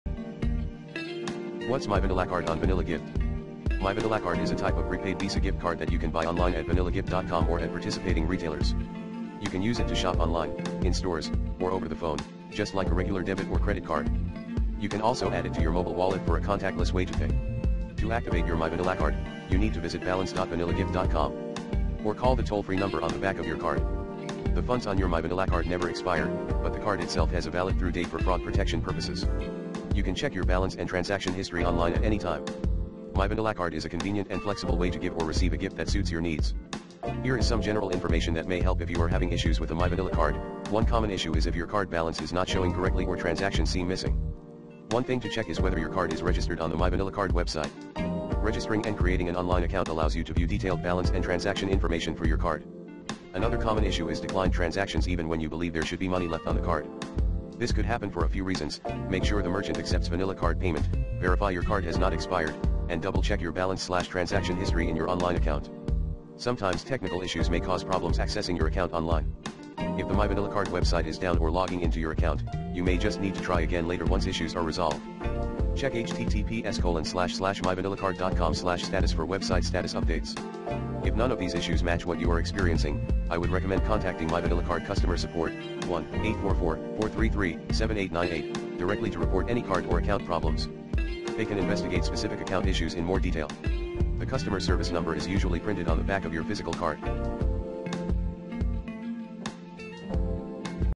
What's myvanillacard on Vanilla Gift? MyVanillaGiftCard?
Find out What myvanillacard on Vanilla Gift is.

i. A Vanilla card is a prepaid debit card that can be used for online and in-store purchases. To sign in your Vanilla card, you need to activate it first and create an online account.

To activate your Vanilla card, you can visit [balance.vanillagift.com] or call the toll-free number that's located on the back of your card: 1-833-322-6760. You'll need to enter the 16-digit card number, expiration date, and CVV for both of these methods.

To create an online account, you can and click on the Sign In button. Then, click on the Create Account link and follow the instructions to register your card and set up your username and password.

If you have already activated your card and created an online account, but you still can't sign in, you may need to check the following:

- Are you entering the correct username and password? Make sure you do not make any typos or use the wrong case. If you forgot your username or password, you can click on the Forgot Username or Password link and follow the steps to reset them.
- Are you using a compatible browser and device? Make sure you are using a supported browser, such as Chrome, Firefox, Safari, or Edge, and that your device has a stable internet connection. You may also need to clear your browser cache and cookies, or try a different browser or device.
- Is your card valid and active? Make sure your card has not expired or been deactivated. You can check your card balance and status by visiting [balance.vanillagift.com] or calling the toll-free number on the back of your card.

If none of these solutions work, you may need to contact the customer support team for further assistance.

ii. Vanilla Gift cards are prepaid cards and do not function as traditional debit cards with accounts to manage. They don't have features like account statements, overdraft protection, or direct deposit. However, I can still offer some tips to help you make the most of your Vanilla Gift card:

**Before you swipe:**

* **Activate your card:** This is crucial! Visit balance.vanillagift.com or call the toll-free number on the back to activate your card before making any purchases.
* **Check your balance:** Knowing how much you have to spend is essential. You can check your balance online or through the phone number on the back.
* **Plan your purchases:** Vanilla Gift cards can't be reloaded, so spend them wisely. Plan your purchases to ensure you stay within your budget and don't waste any remaining funds.

**Using your card:**

* **Choose "credit" at checkout:** While your Vanilla Gift card technically functions like a debit card, selecting "credit" at checkout avoids potential transaction fees associated with debit purchases.
* **Set a PIN (optional):** You can set a four-digit PIN for added security, especially for online purchases.
* **Beware of fees:** Some merchants may charge transaction fees for using prepaid cards. Look out for any signage or ask beforehand to avoid surprises.
* **Keep receipts:** Save your receipts for tracking your spending and in case of any disputes or returns.

**Maximizing value:**

* **Use them for online subscriptions:** Paying for streaming services, music subscriptions, or online storage with your Vanilla Gift card is a great way to avoid linking your bank account or credit card.
* **Split purchases:** If you're making a large purchase with friends or family, consider using your Vanilla Gift card to cover part of the cost and splitting the remaining amount with another payment method.
* **Buy gift cards:** Use your Vanilla Gift card to purchase gift cards for other stores or restaurants, essentially extending its purchasing power to a wider range of options.
* **Combine with sales and discounts:** Look for deals and promotions at your favorite stores to stretch the value of your Vanilla Gift card even further.

**Safety tips:**

* **Never share your card information:** Treat your Vanilla Gift card like any other debit or credit card. Don't share its details online, over the phone, or with anyone you don't trust.
* **Beware of scams:** Phishing scams targeting prepaid cards are common. Be cautious of unsolicited emails or calls asking for your card information.
* **Report lost or stolen cards immediately:** If you lose your card or suspect fraudulent activity, contact Vanilla Gift customer service right away to block further transactions and potentially get a replacement card.